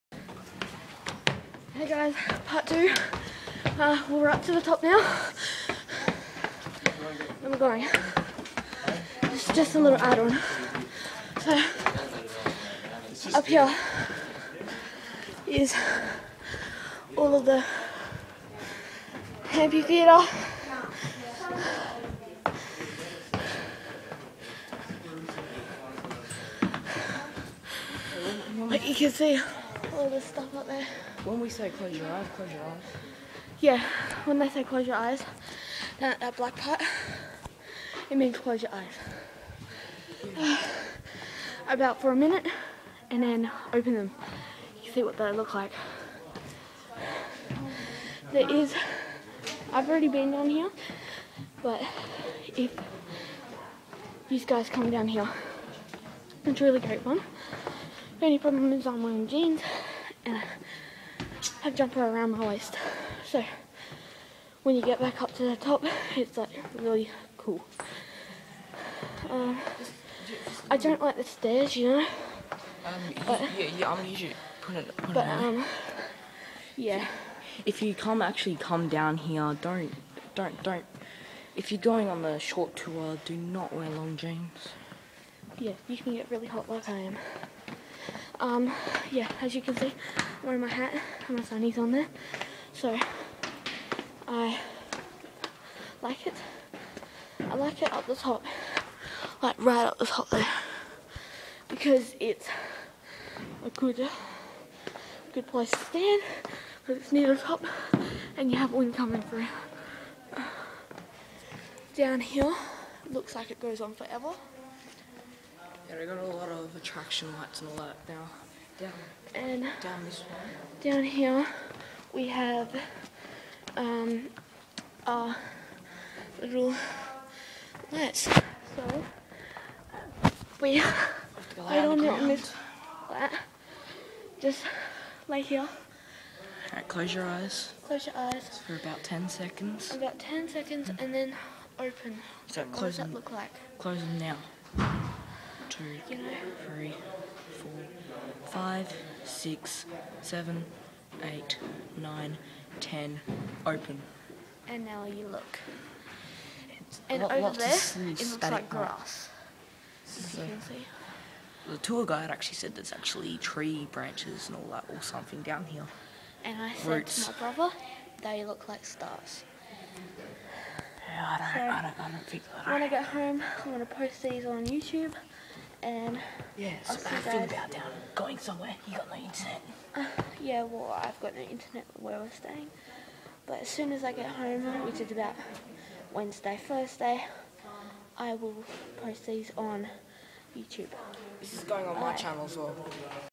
Ngilgi cave part 2
This is the other side of the cave called the amphitheater . The first part was about the main chamber